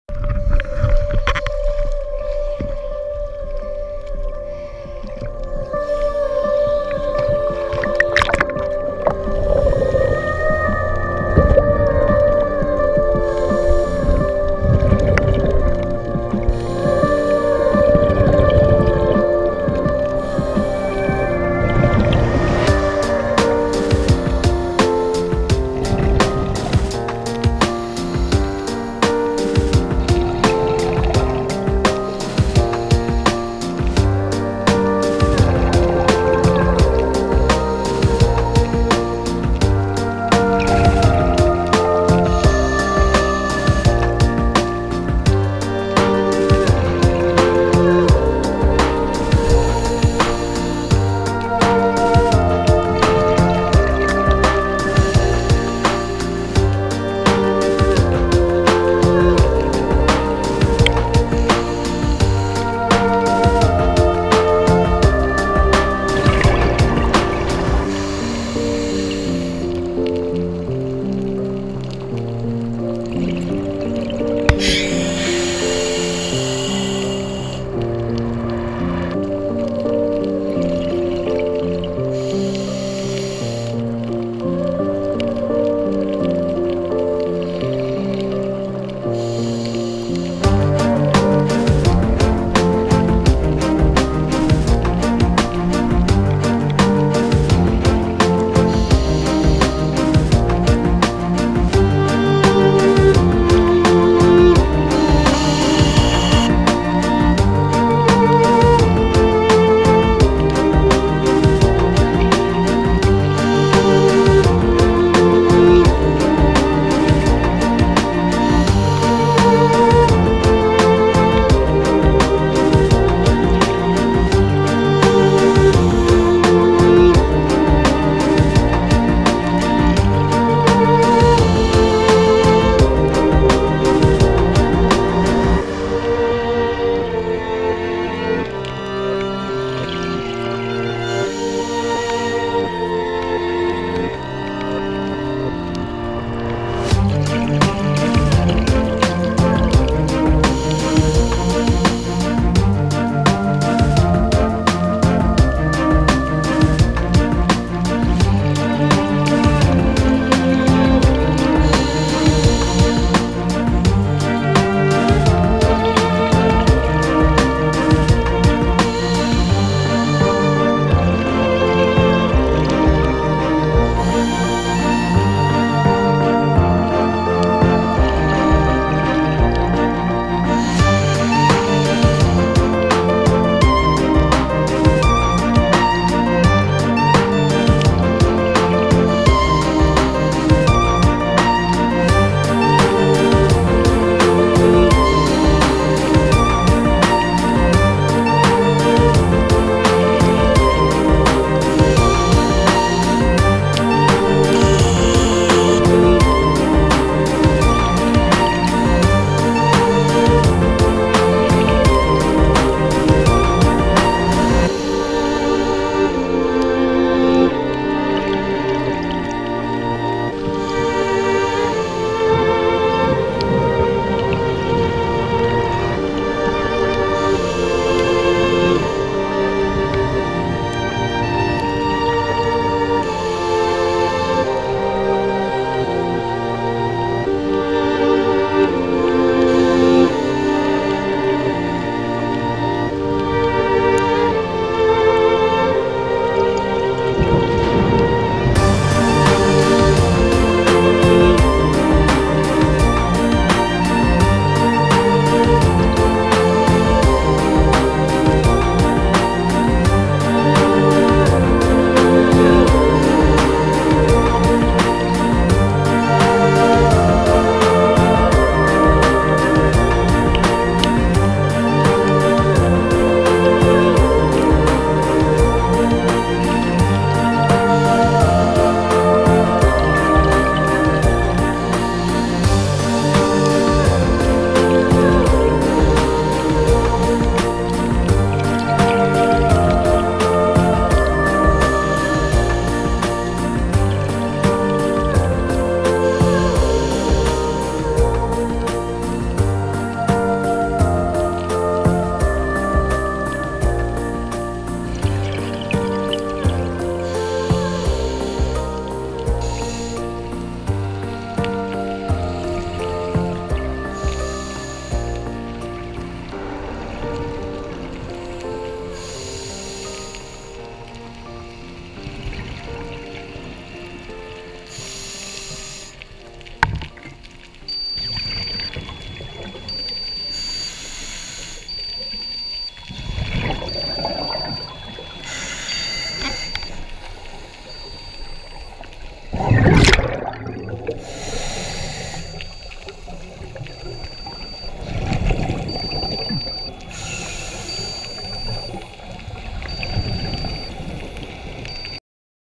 Sportfisher Reef Dive
A deep dive with my coworkers to explore a local fishing reef called the 'Sportfisher' in the San Pedro Channel -Los Angeles, CA.

September, 2015.